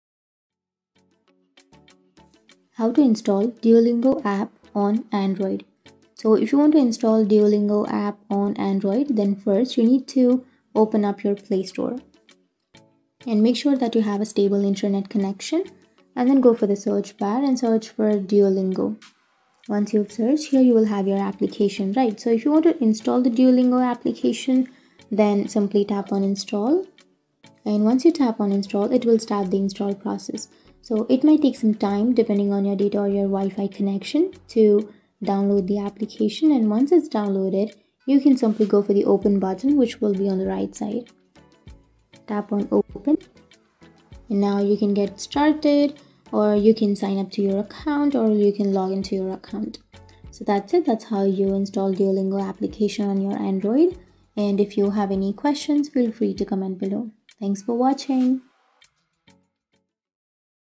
How To Install Duolingo App On Android
Discover the easy steps to install the Duolingo app on your Android device with QuickFixTutorials. Our step-by-step guide will assist you in downloading and setting up the Duolingo language learning app, helping you embark on a journey to enhance your language skills.